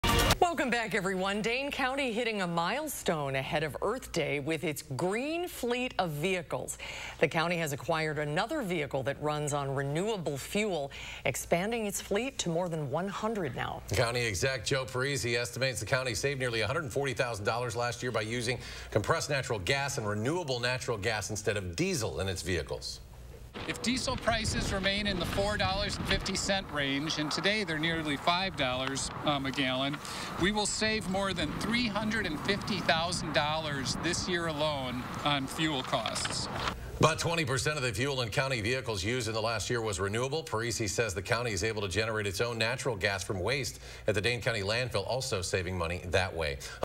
Dane Co. saving hundreds of thousands of dollars annually with natural gas-fueled vehicles
Dane County Executive Joe Parisi announced Thursday the county has acquired another vehicle that runs on renewable fuel, expanding its fleet to 100 RNG and RNG/gas hybrid vehicles, to go with its 3 electric vehicles and 13 hybrid electric vehicles.